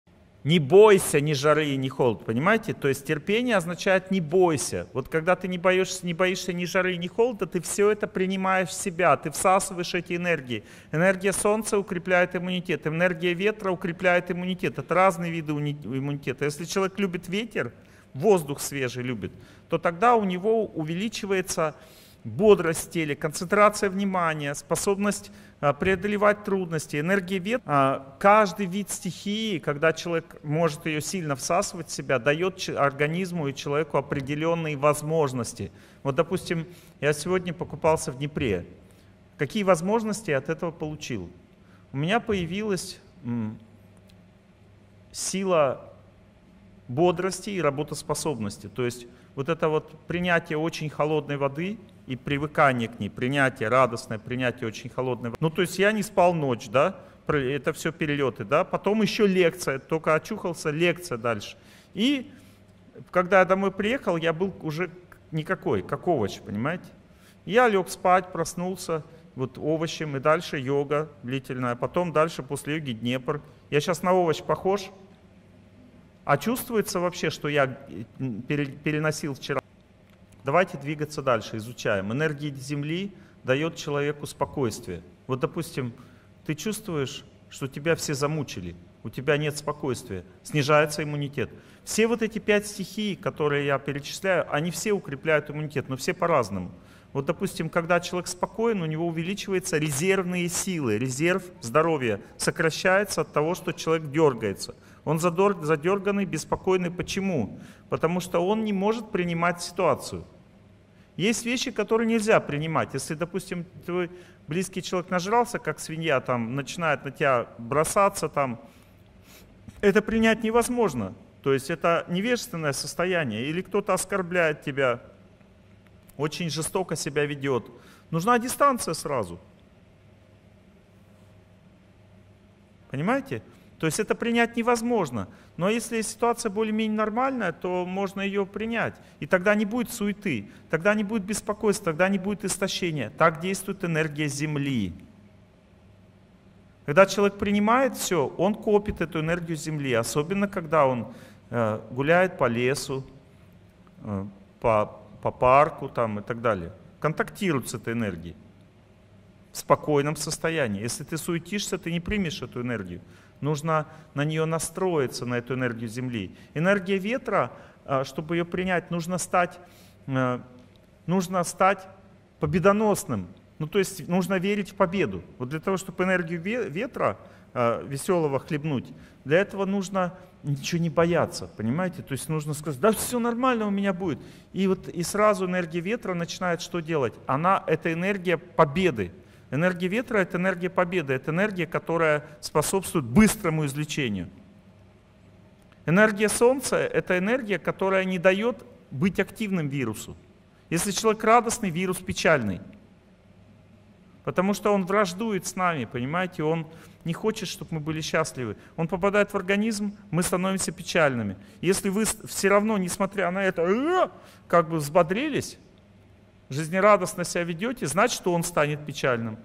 Торсунов О.Г. Про энергию
В этом видео Олег Геннадьевич Торсунов говорит про энергию
Из лекции «Мировой кризис и моя жизнь»,  Киев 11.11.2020